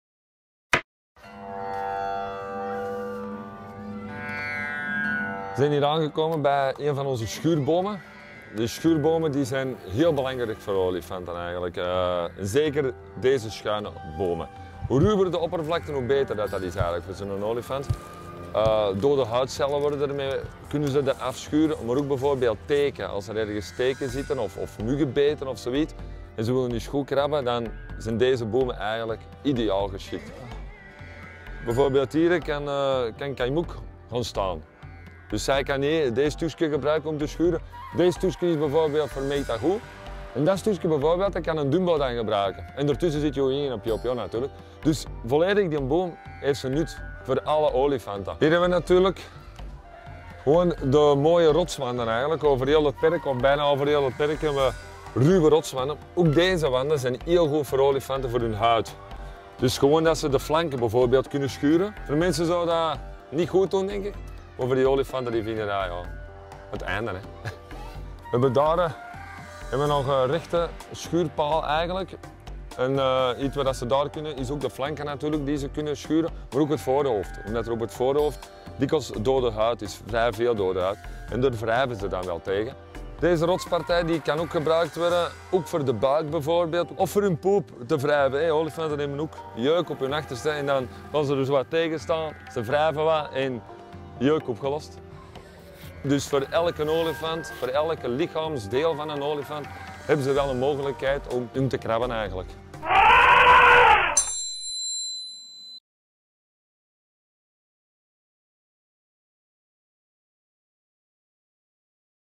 Sneakpreview 9: Jeuk? Op naar de scrubboom!
Speciale bomen en ruwe rotswanden op maat van elke olifant om heerlijk tegen aan te schurken. Aan wellness geen gebrek in het nieuwe olifantenverblijf in Planckendael! Verzorger Tony laat zien hoe het moet.

Tot en met de grote opening van de olifantentempel op 29 juni, gunnen we jullie elke dag een blik achter de schermen. Volg onze verzorgers en wandel mee door  het nieuwe paradijs van Kai-Mook, May Tagu, Phyo Phyo, YuYuYin en Dumbo!